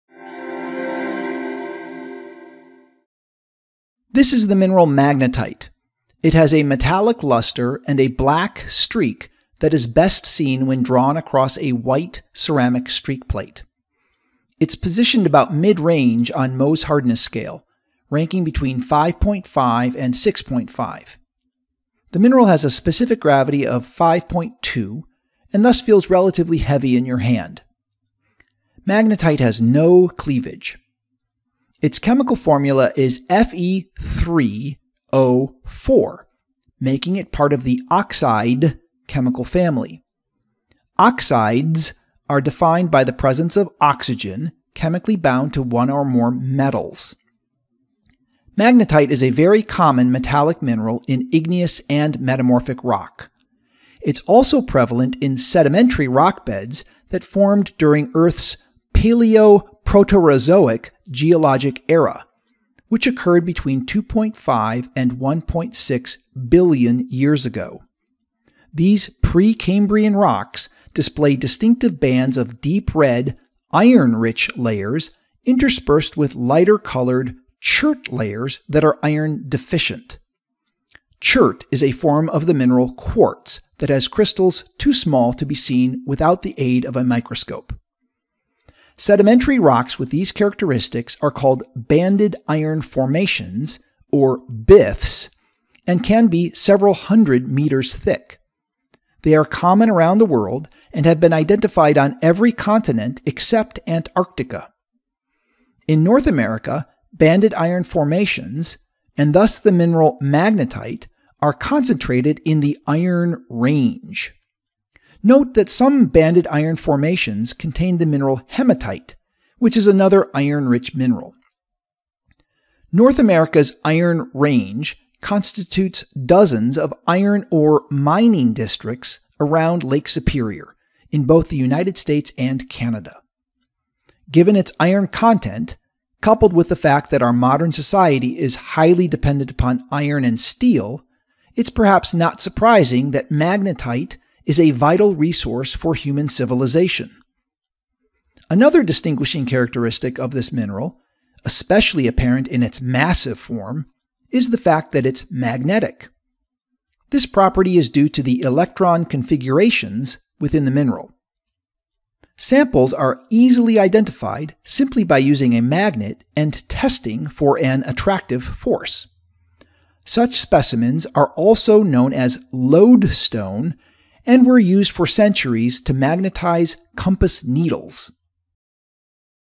Magnetite
This is the mineral Magnetite. It has a metallic luster and a black streak that is best seen when drawn across a white ceramic streak plate. It’s positioned about mid-range on Mohs hardness scale, ranking between 5.5 and 6.5. The mineral has a specific gravity of 5.2 and thus feels relatively heavy in your hand. Magnetite has no cleavage. Its chemical formula is Fe3O4, making it part of the Oxide chemical family. Oxides are defined by the presence of Oxygen chemically bound to one or more metals. Magnetite is a very common metallic mineral in igneous and metamorphic rock. It’s also prevalent in sedimentary rock beds that formed during Earth’s Paleo-Proterozoic Geologic Era, which occurred between 2.5 and 1.6 billion years ago. These Pre-Cambrian rocks display distinctive bands of deep red, Iron rich layers interspersed with lighter colored Chert layers that are iron deficient. Chert is a form of the mineral Quartz that has crystals too small to be seen without the aid of a microscope. Sedimentary rocks with these characteristics are called Banded Iron Formations, or BIFs, and can be several hundred meters thick. They are common around the world and have been identified on every continent except Antarctica. In North America, Banded Iron Formations, and thus the mineral Magnetite, are concentrated in the Iron Range. Note that some Banded Iron Formations contain the mineral Hematite, which is another Iron-rich mineral. North America’s Iron Range constitutes dozens of Iron ore mining districts around Lake Superior, in both the United States and Canada. Given its Iron content, coupled with the fact that our modern society is highly dependent upon Iron and Steel, it’s perhaps not surprising that Magnetite is a vital resource for human civilization. Another distinguishing characteristic of this mineral, especially apparent in its massive form, is the fact that it’s magnetic. This property is due to the electron configurations within the mineral. Samples are easily identified simply by using a magnet and testing for an attractive force. Such specimens are also known as Lodestone and were used for centuries to magnetize compass needles.